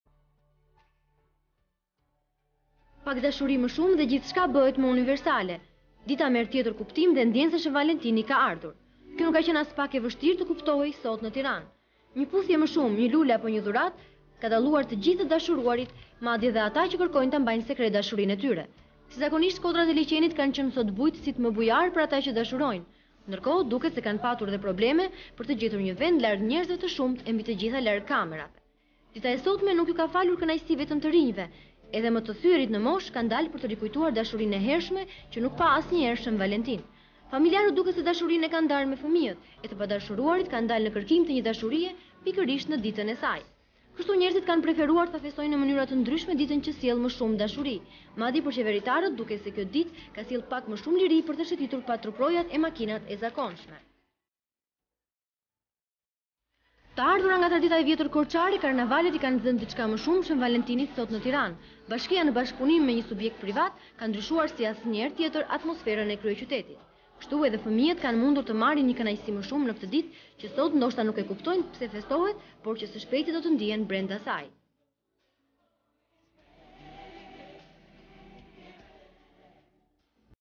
Shen Valentini ne Tirane (14 Shkurt 1999)
Arkiva - Tv Klan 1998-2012; Pamje nga Liqeni i Tiranes, njerez qe festojne Shen Valentinin, duke shetitur ne kembe ose me varke ne liqen.  Pamje nga Bulevardi ku  festohen Karnavalet.

Pak dashuri më shumë dhe gjithçka bëhet universale. Dita merr tjetër kuptim dhe ndjen që Shën Valentini ka ardhur. Një puthje më shumë, një lule apo një dhuratë, ka dalluar të gjithë të dashuruarit në Tiranë.
Kodrat e Liqenit të Tiranës kanë qënë sot bujtësit me bujarë për ata që dashurojnë. Dita e Shën Valentinit nuk u ka falur kënaqësi vetëm të rinjve, edhe më të thyerit në moshë kanë dalë për të rikujtuar dashurinë e hershme. Familjarët, dashurinë duket e kanë ndarë me fëmijët.
Njerëzit kanë preferuar të festojnë në mënyra të ndryshme ditën që sjell më shumë dashuri.  Për qeveritarët duket se kjo ditë ka sjellë pak më shumë liri për të shëtitur pa truprojat e makinat e zakonshme. Karnavalet i kanë dhënë diçka më shumë Shën Valentinit në Tiranë. Bashkia me një subjekt privat ka ndryshuar atmosferën e kryeqytetit.